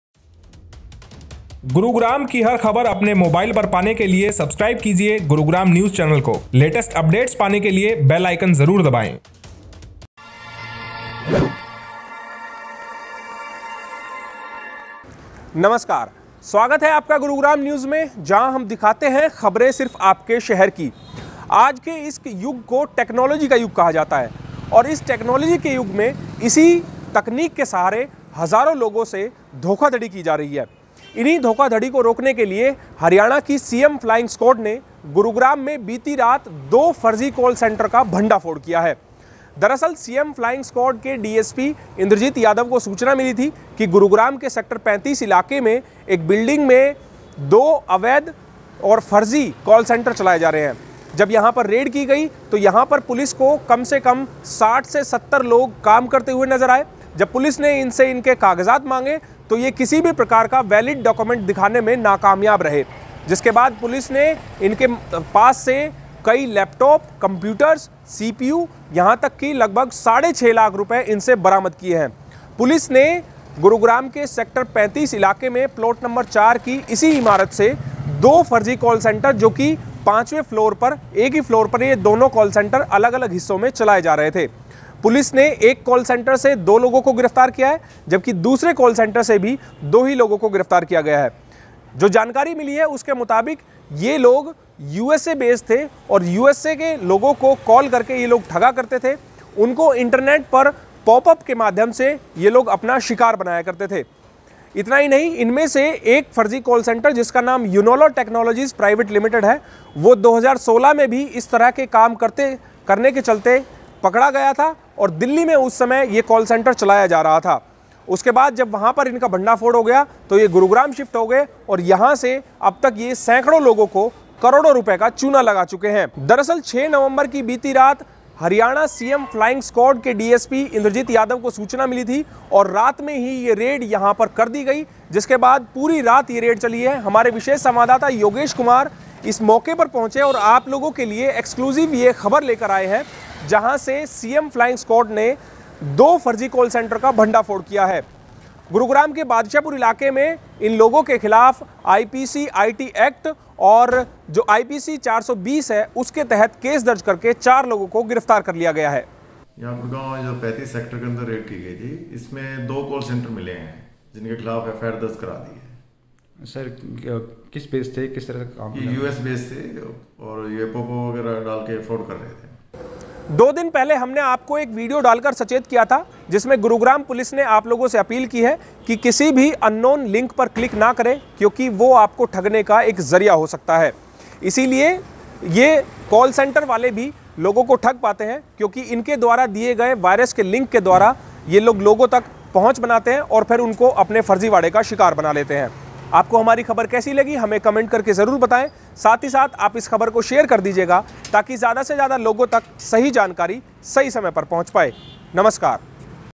Gurugram News - सीएम फ्लाइंग ने दो फर्ज़ी कॉल सेंटर्स का किया भंडाफोड़
सीएम फ्लाइंग स्कॉयड ने गुरुग्राम में दो फर्जी कॉल सेंटर का भंडाफोड़ किया है । जो कि गुरुग्राम के सेक्टर 35 इलाके में एक ही बिल्डिंग से ऑपरेट किए जा रहे थे । पुलिस ने दोनों कॉल सेंटर से चार लोगों को गिरफ्तार किया है ।